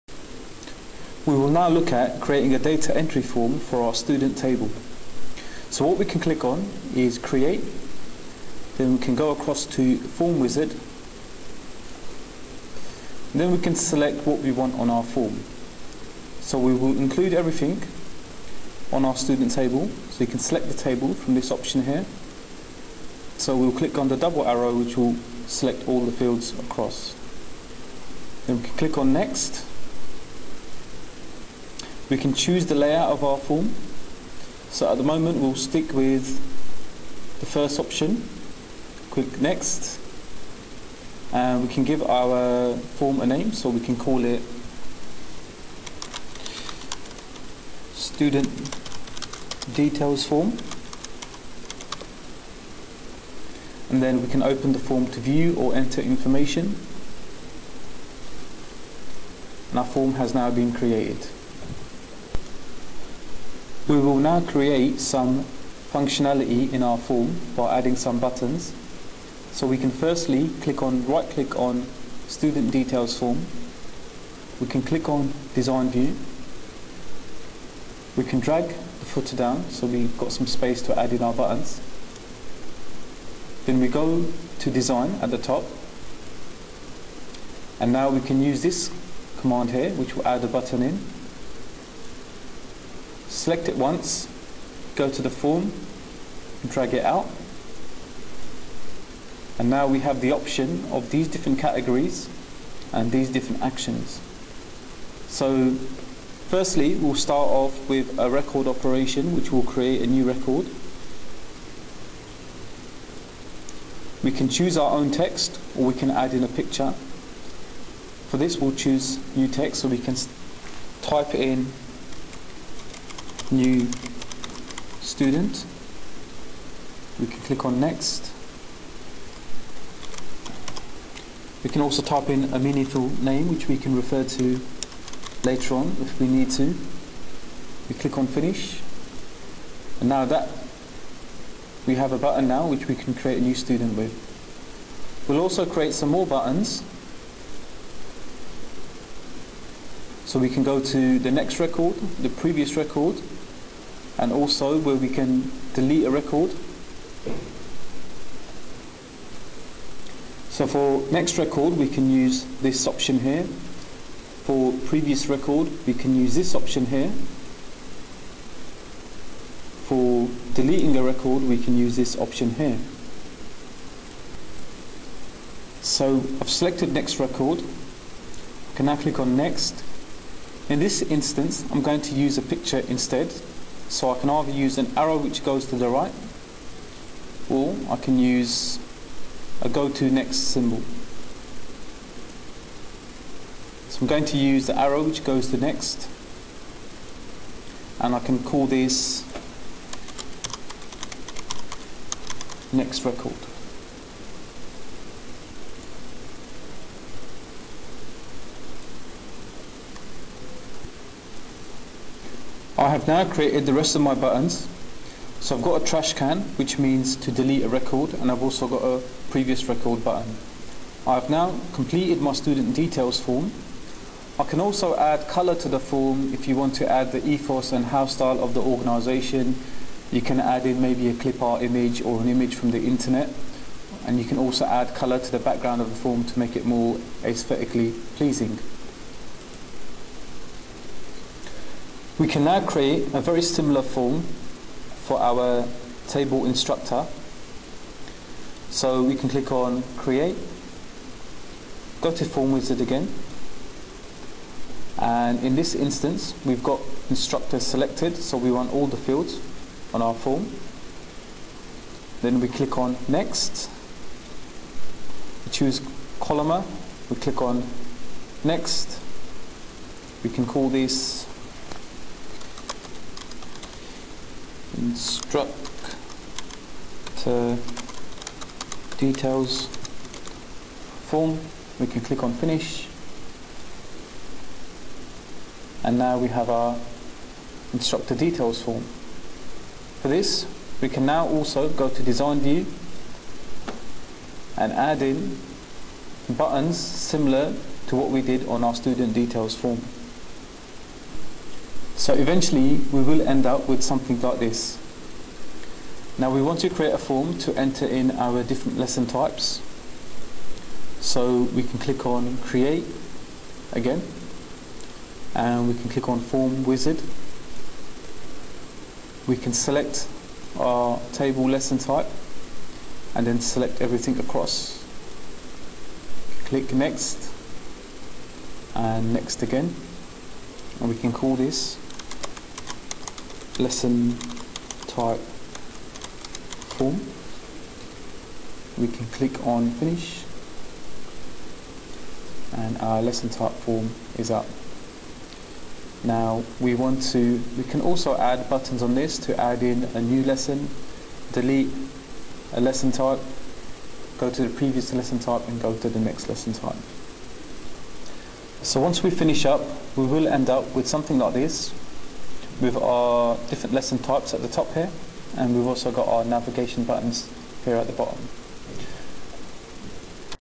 Creating a relational database in access 2013 Part 2
In this video you will learn how to create forms and buttons using Microsoft Access 2013. In the next video we will look at creating a form with a subform.